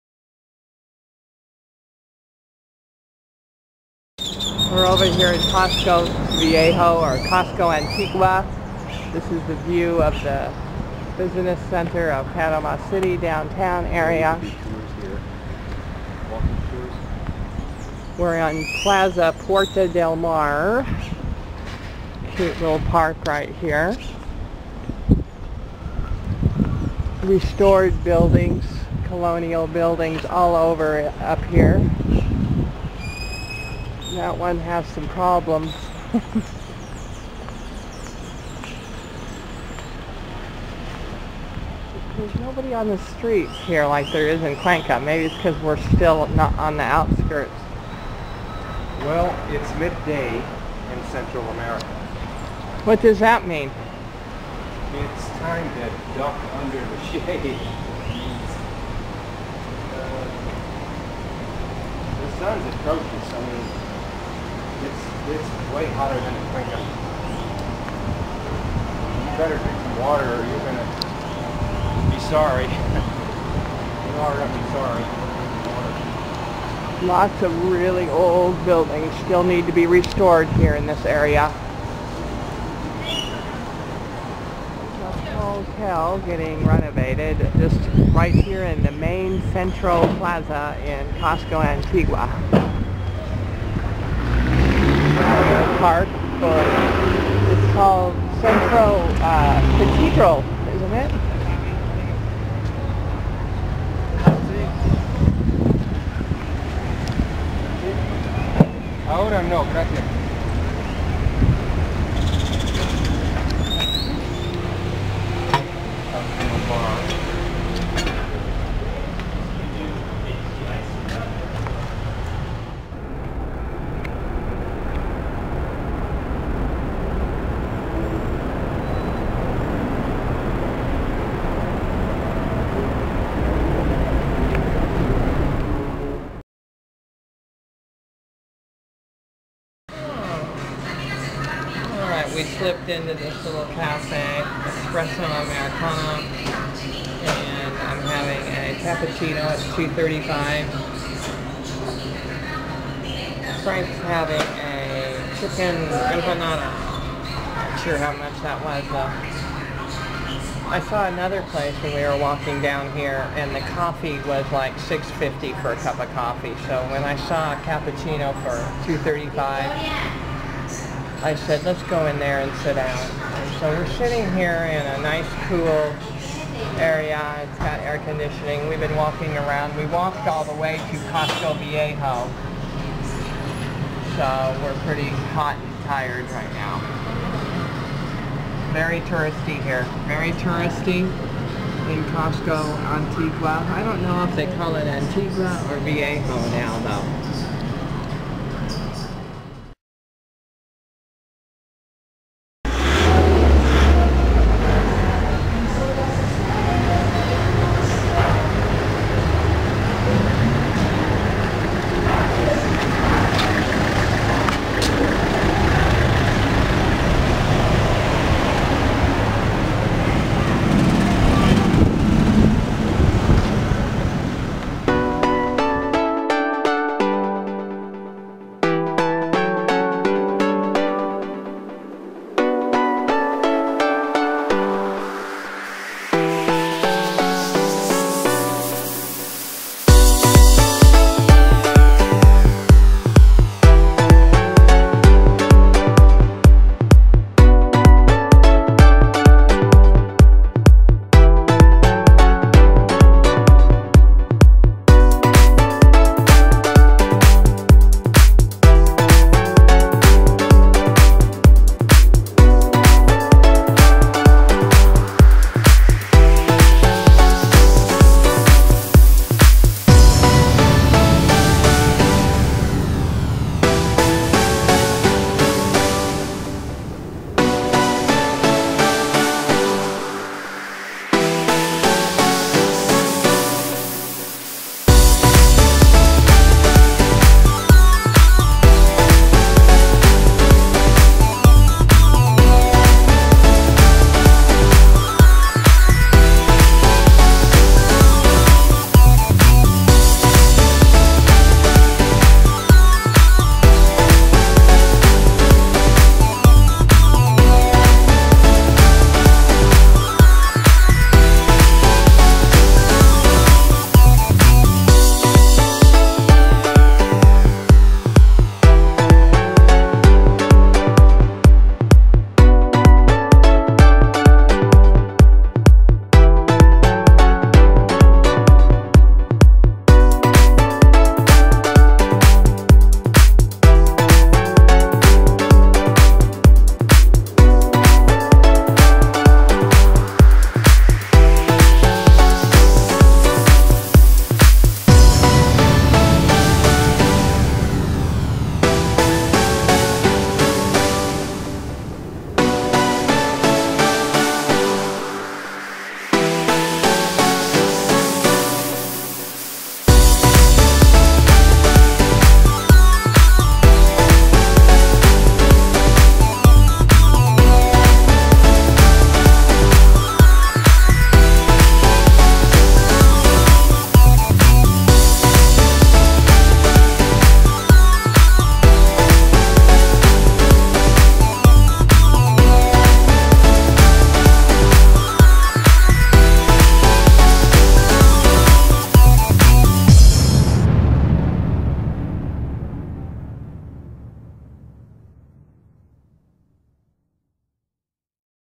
Beautiful Casco Antigua Neighborhood of Panama City, Panama
We spent the day in Casco Antigua in Panama City, Panama.